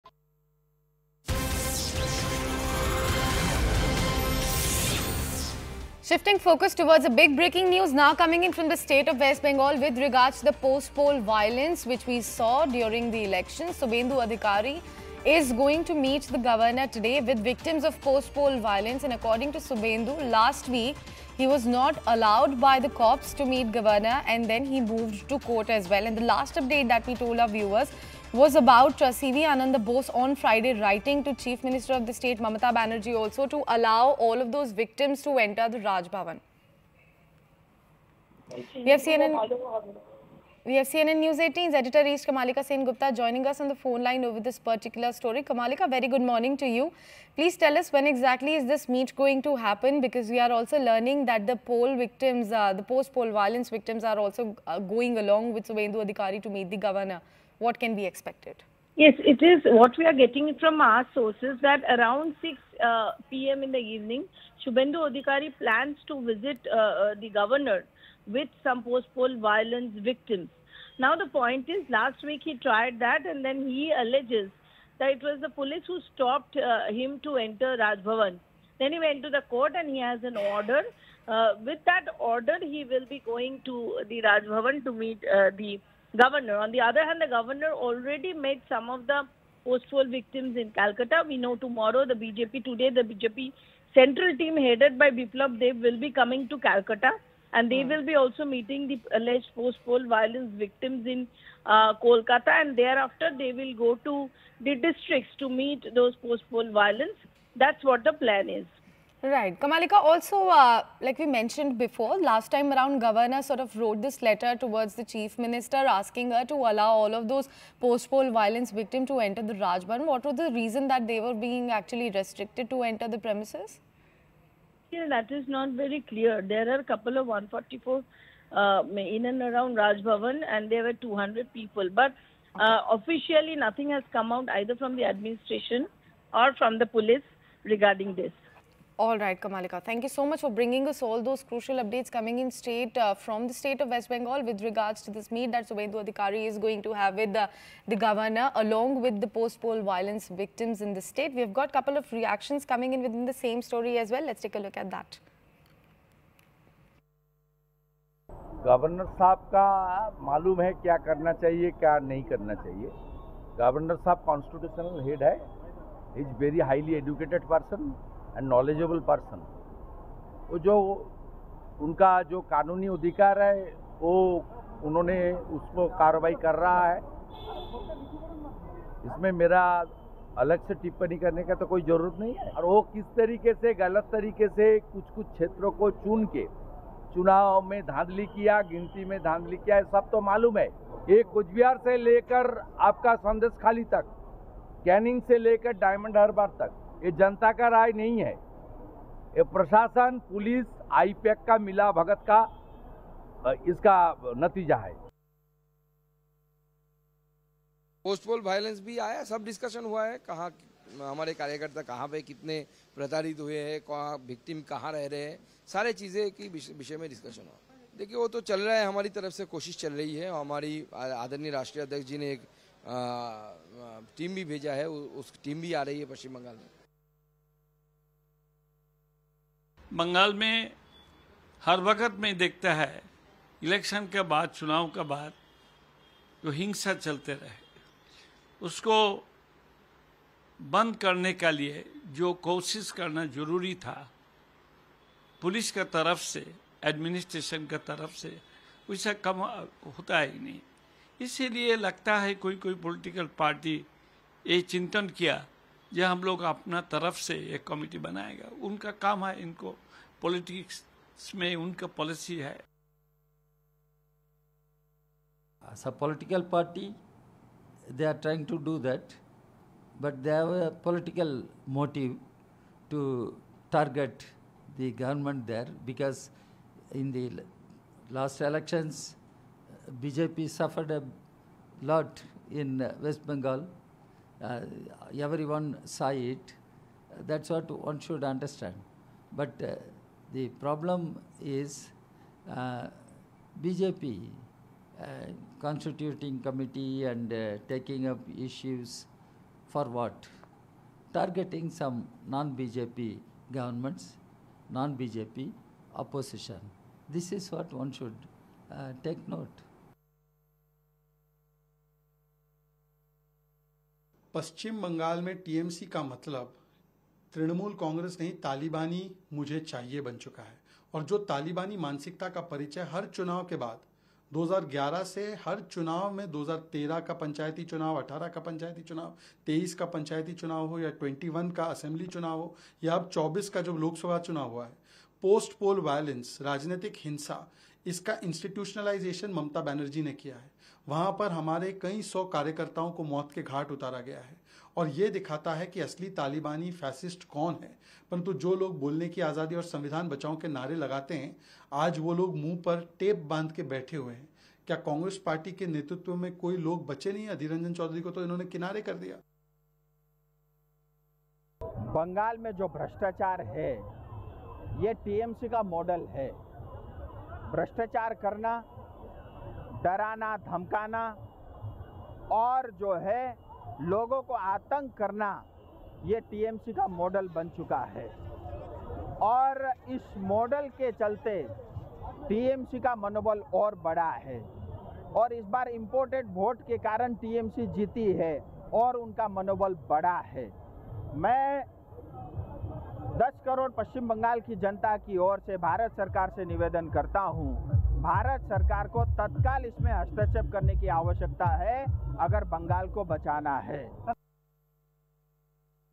Suvendu Adhikari To Meet West Bengal Governor With Victims Of Post Poll Violence In Bengal | News18
BreakingNews | Suvendu Adhikari to meet the West Bengal Governor with victims of post poll violence in Bengal.

n18oc_india